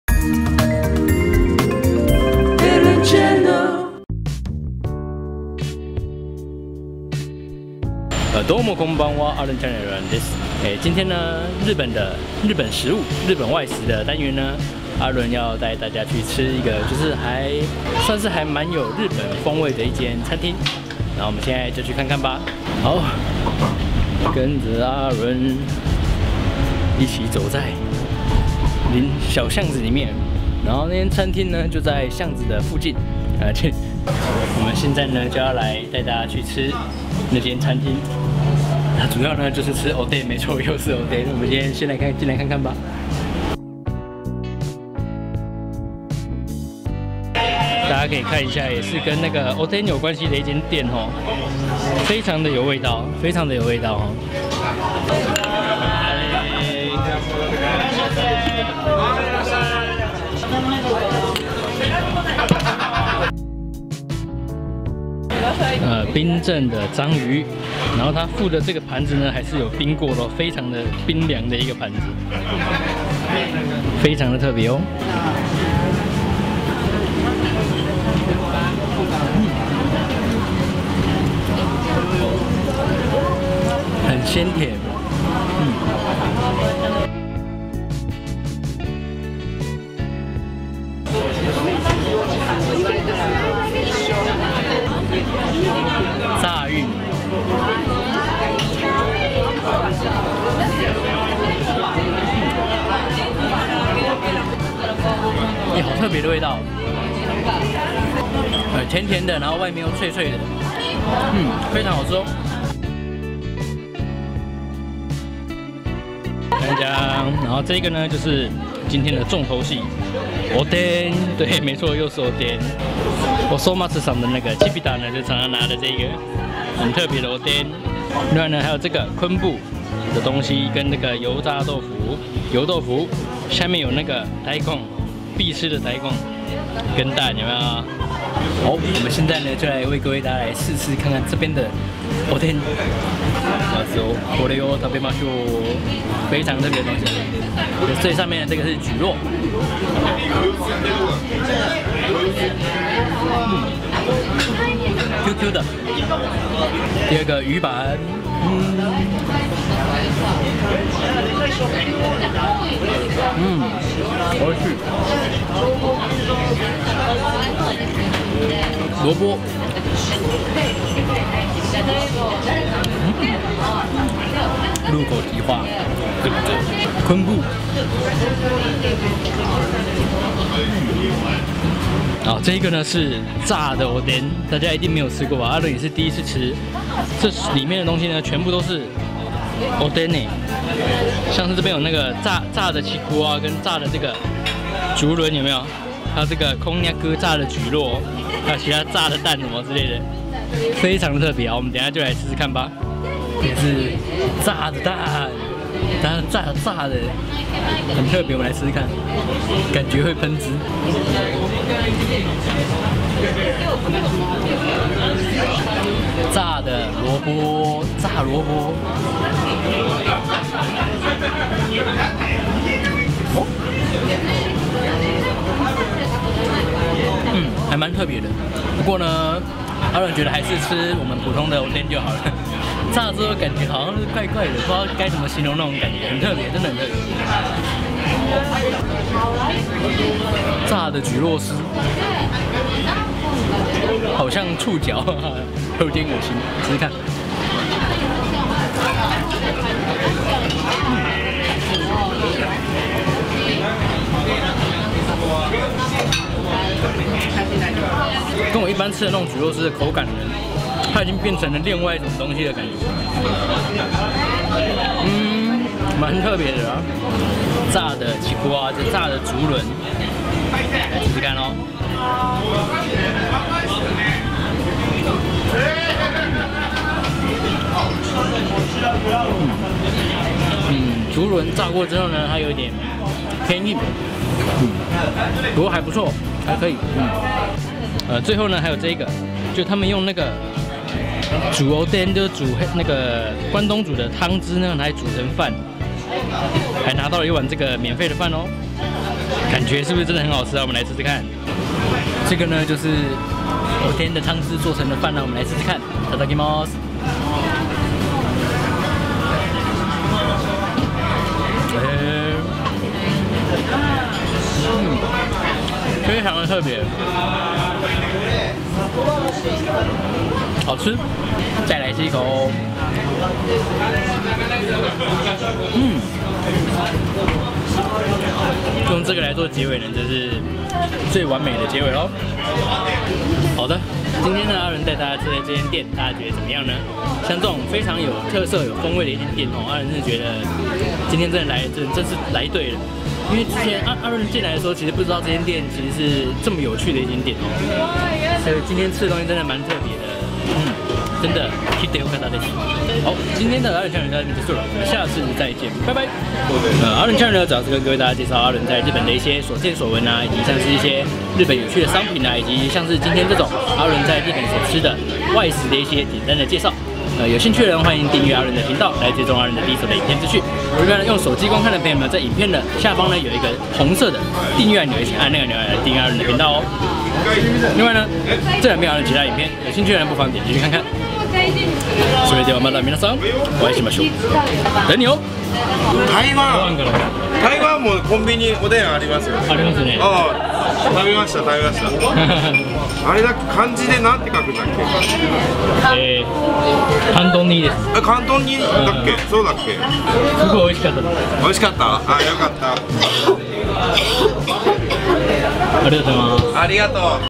日本神奈川超特別關東煮居酒屋 あっかん《阿倫來吃喝》 跟著阿倫一起吃日本#5
又到了日本外食的時間啦！
這次阿倫來到了神奈川縣的逗子的一間很特別的居酒屋。
他主要是主打日式關東煮的一間居酒屋，
而且最特別的是他居然還有「炸關東煮」跟「炸玉米」兩道阿倫從來沒聽過也沒吃過的料理，
重點是還很好吃…
一起來看看吧

※店家資訊※
【おでん酒場-あっかん】
神奈川県逗子市逗子5-1-12
カサハラビルＢ棟3階303

■日本女生會想跟台灣男生交往嗎？日本街頭大驗證

■日本Suica卡跟PASMO卡(悠遊卡/八達通)的購買與儲值方法